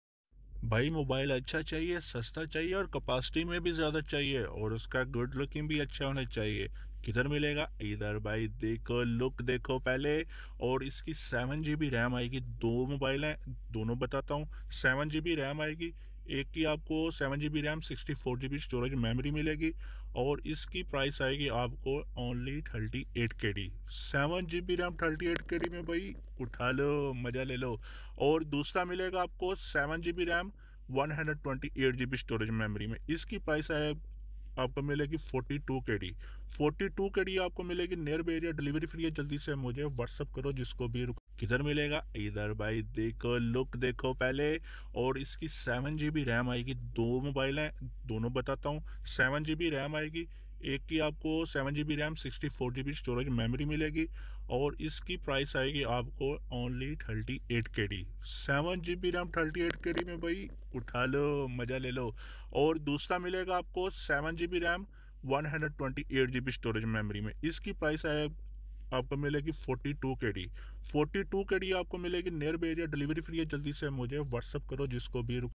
Infinix Hot 20i 7GB 128GB 5000Mah
Infinix Hot 20i comes with a 6.6 inch Display
with 720x1612 pixels of resolution
Sports 8-cores CPU
Android 12 Opertaing System
With 13 MP main Camera and 8 MP Selfie Camera
5000mah Battery
7gb 64gb 38KD
7gb 128gb 42KD
🚙Free Home Delivery Near Erea🚙
⌨️Leave your Message on Whatsapp We Reply Soon⌨️